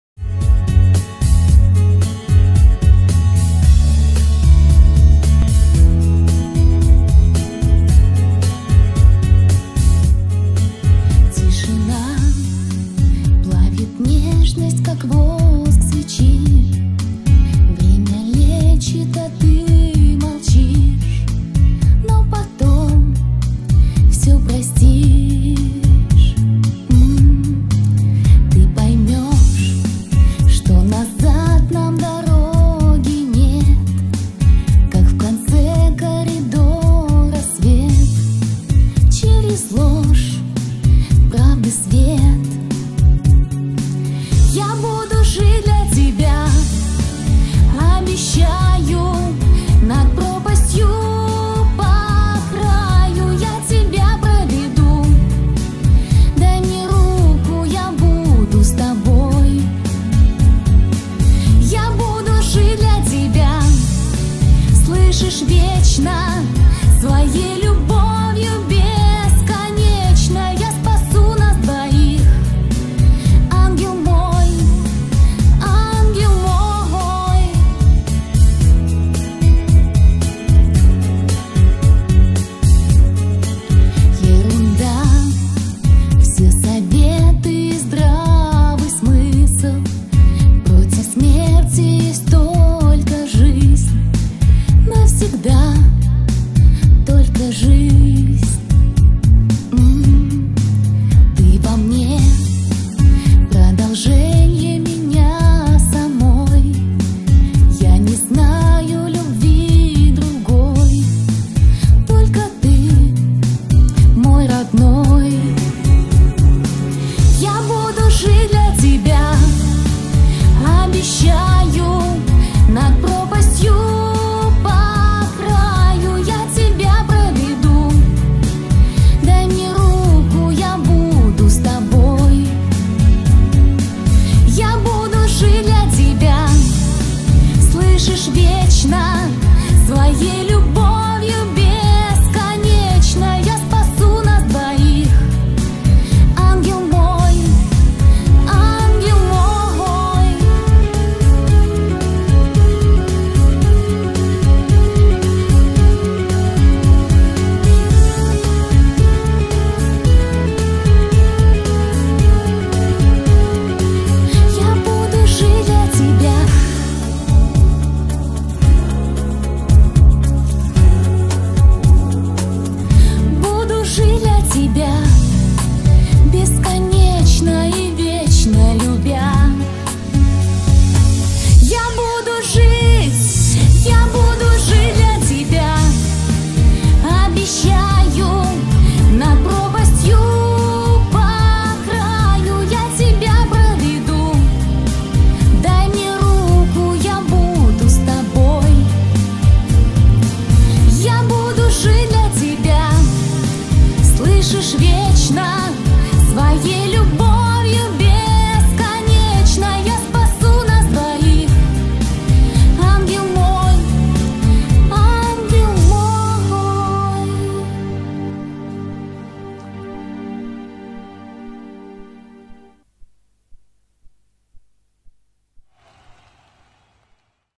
Roman & Xenia Bride's Song
Cвадебное видео. Запись фонограммы - студия NEW TONE Н.Новгород